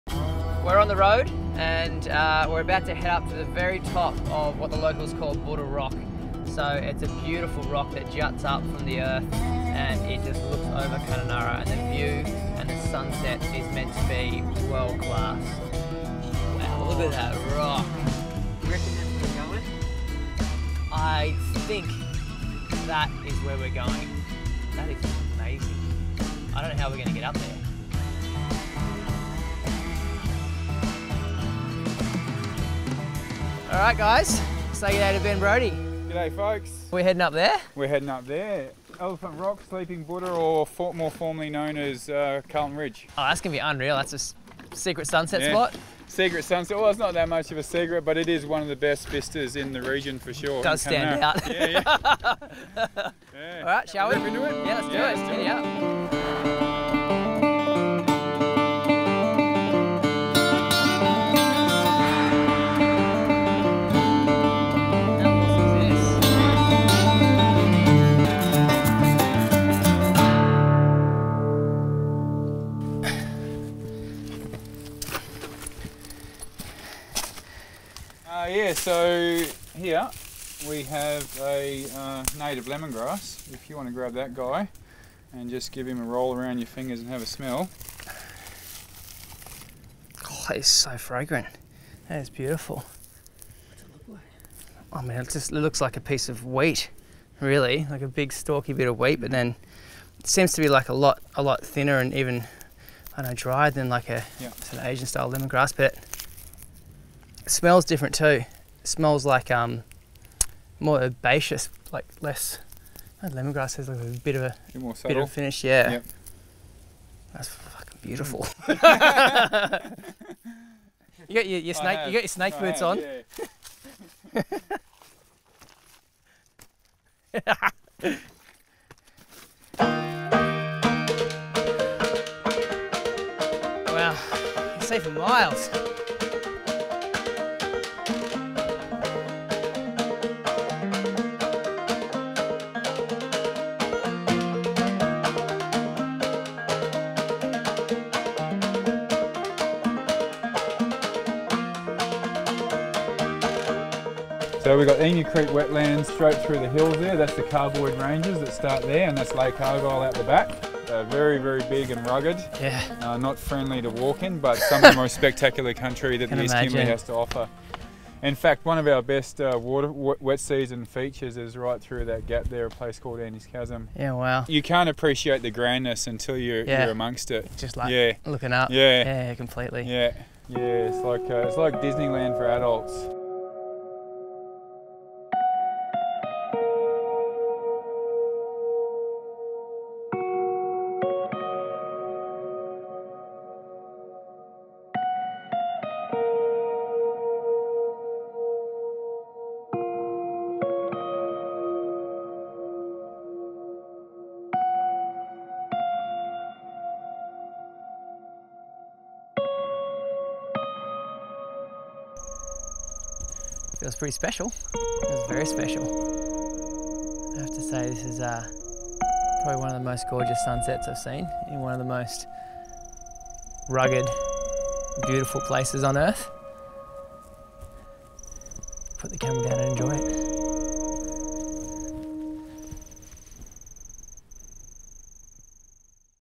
CLIMBING ELEPHANT MOUNTAIN FOR A KIMBERLEY SUNSET | Bondi Harvest Journeys Western Australia
The best sunset Australia has to offer !!! We connected with photographer and local legend Ben Broady to take us to his favourites view point of Kununurra elephant rock. We had no idea what we were getting ourselves into but we knew it would be an adventure of a lifetime. A quick boat ride followed by a 45-degree angle rock climb in 30 degree heat and we were at the top and I can tell you now it was well worth the effort WOW ! 10/10 perfection